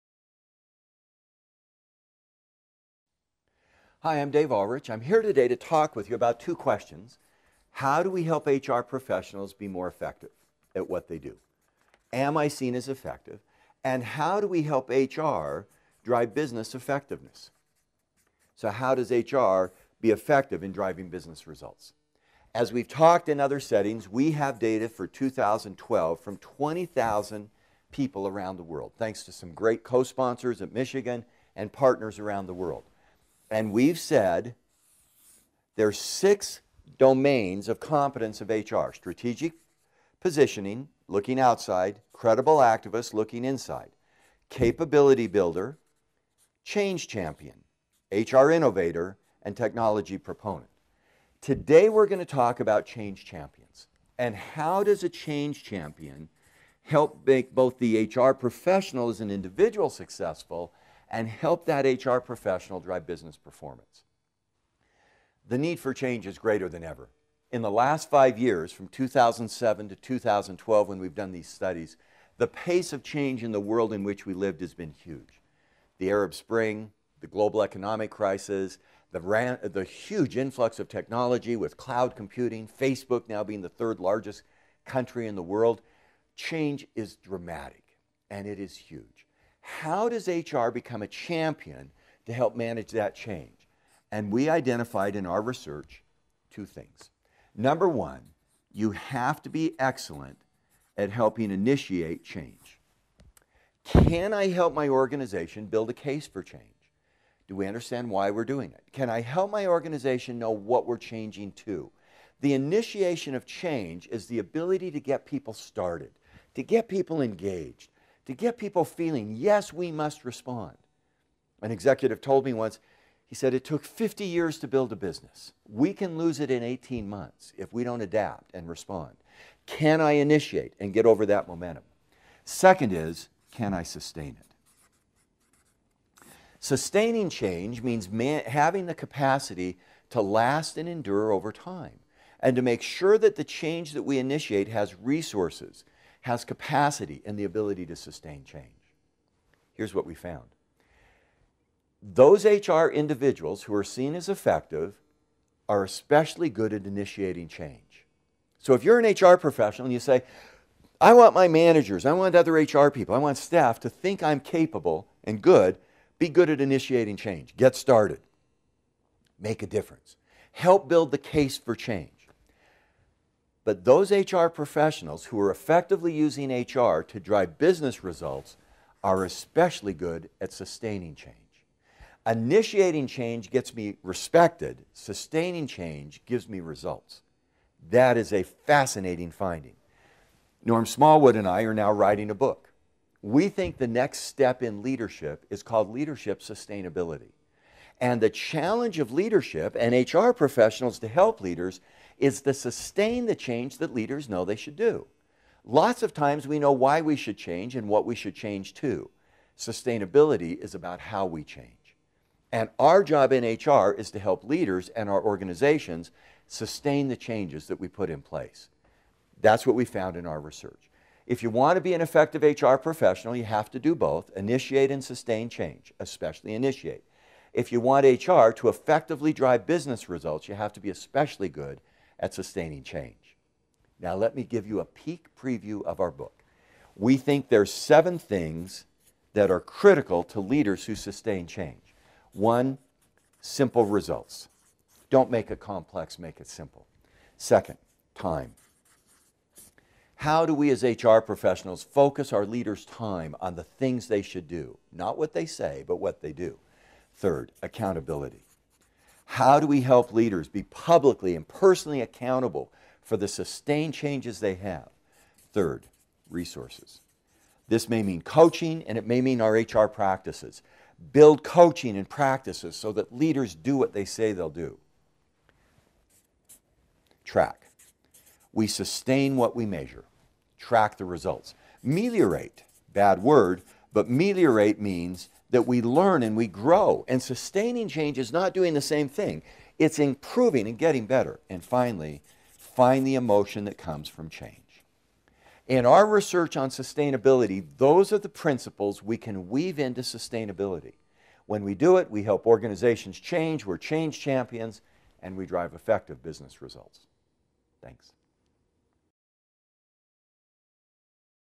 Change Champion - HR Competency Study
Dave Ulrich explains how to become a Change Champion, one of the competencies from the 2012 round of the HR Competency Study. HRCS is the longest running, most comprehensive study in the HR profession.

The results revealed 6 critical competencies for all HR professionals:
Strategic Positioner
Credible Activist
Capability Builder
Change Champion
HR Innovator and Integrator
Technology Proponent

To strengthen these competencies for HR professionals in your organization, inquire